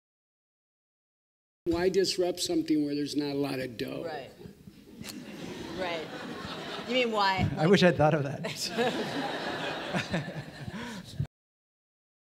Wait a Minute, Why Disrupt: Serial and the Podcast Explosion | Journalism+Design
Sarah Koenig, Alex Blumberg, Benjamin Walker, and Alix Spiegel with David Carr laugh about the lack of money in the Podcast Model.

The Journalism and Design Program at Eugene Lang College The New School for Liberal Arts is proud to host this conversation with David Carr of The New York Times and Lack Professor of Media Studies at Boston University moderating a discussion with Sarah Koenig, host and producer of "Serial," Alex Blumberg, creator of the podcast "StartUp," former producer of "This American Life," and founder of NPR's "Planet Money," Benjamen Walker, host of the podcast "Theory of Everything" and one of the founding members of Radiotopia, and Alix Spiegel, a founding producer of "This American Life" and co-host of the New NPR series "Invisibilia," about the unprecedented popularity of "Serial," the creative explosion currently taking place in podcasting, and what it all means for the industry.

Since its October debut, "Serial" has taken the podcasting world by storm. A spinoff of the public radio show "This American Life," "Serial" is now the world's most popular podcast, with more than 5 million downloads and 1.5 million listeners per episode, according to iTunes. While this fame is partially due to the show's intriguing tale of a real-life murder case from 1999, it also speaks volumes about the public's growing interest in podcasts.

Join the conversation on Twitter!

Thursday, February 5, 2015 at 8:00 pm to 10:00 pm
The Auditorium at 66 West 12th Street, Alvin Johnson/J.M. Kaplan Hall